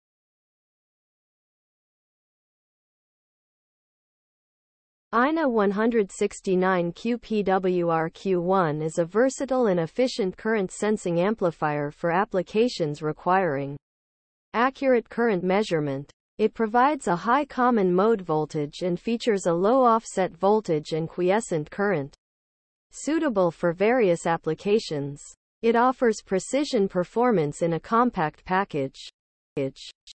INA169QPWRQ1 Power Management ICs - Avaq
The INA139 and INA169 are high-side, unipolar, current shunt monitors. Wide inputcommon-mode voltage range, high-speed, low quiescent current, and tiny SOT-23 packaging enable usein a variety of applications.
Input common-mode and power-supply voltages are independent and can range from 2.7 V to40 V for the INA139 and 2.7 V to 60 V for the INA169. Quiescent current is only 60 μA, whichpermits connecting the power supply to either side of the current measurement shunt with minimalerror.
The device converts a differential input voltage to a current output. This current isconverted back to a voltage with an external load resistor that sets any gain from 1 to over 100.Although designed for current shunt measurement, the circuit invites creative applications inmeasurement and level shifting.
Both the INA139 and INA169 are available in 5-pin SOT-23 packages. The INA139 device isspecified for the –40°C to +125°C temperature range, and the INA169 is specified from –40°C to+85°C.